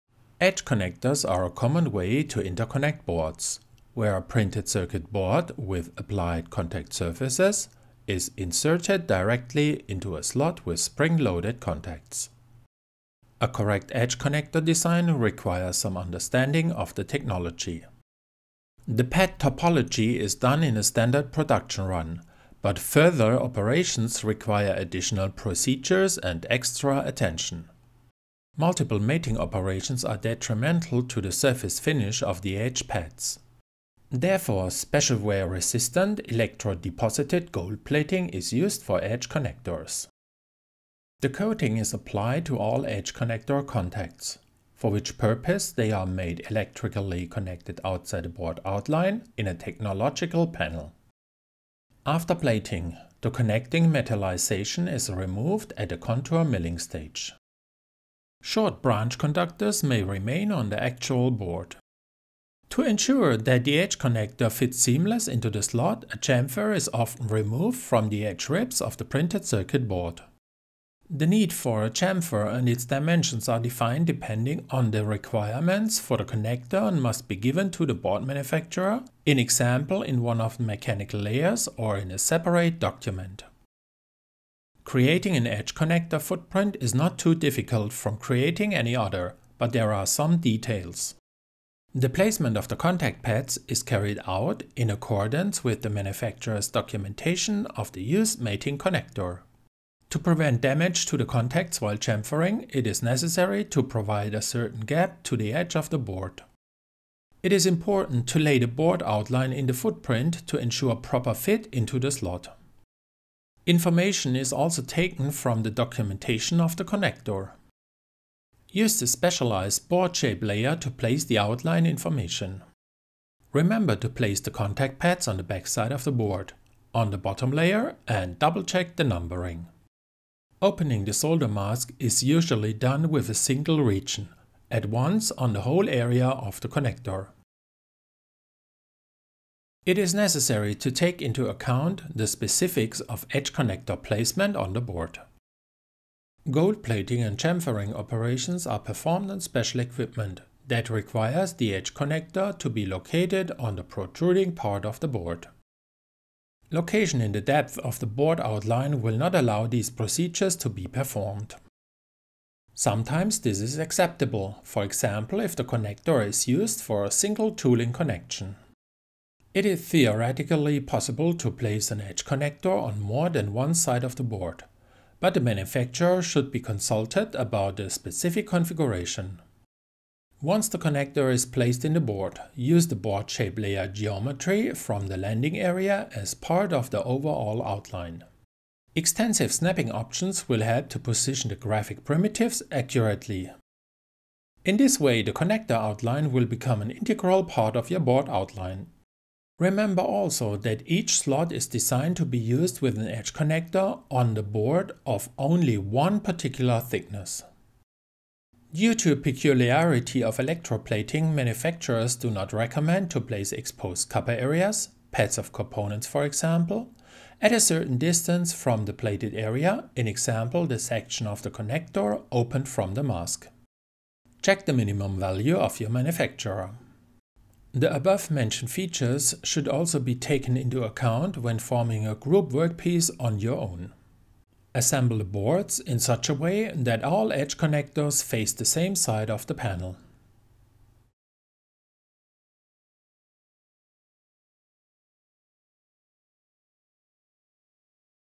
What to Consider When Designing Edge Connectors
If your design requires edge connectors there are a few important things to consider. Learn how to make sure your design with edge connectors is easy and manufacturable.

0:00 Intro
0:52 After PLating
1:02 Edge Connector Overview
1:24 Creating an Edge Connector
3:00 Accurate Positioning